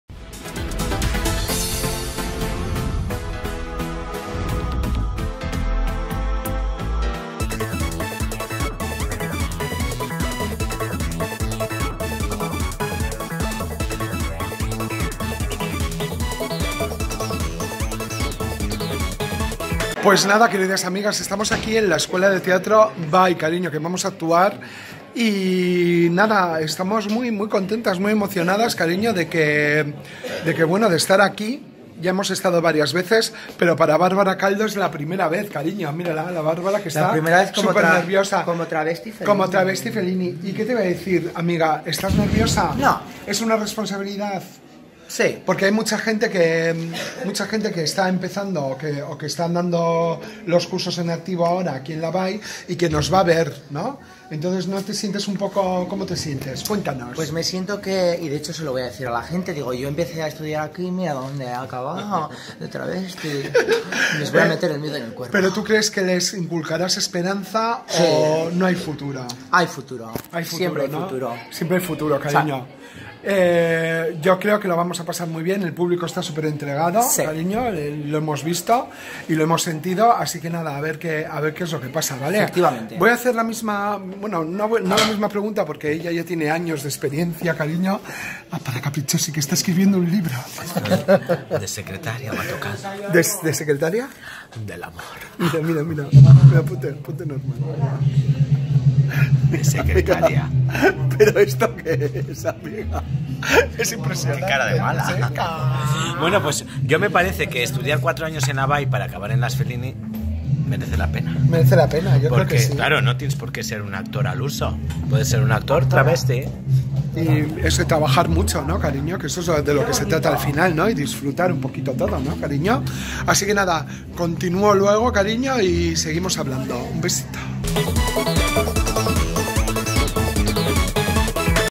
EL DIARIO DE NAGORE: con mis compañeras en la BAI de Barakaldo
Con mis compañeras Barbara y Caprichossi hablando de estudiar en la BAI para acabar como travesti en Las Fellini.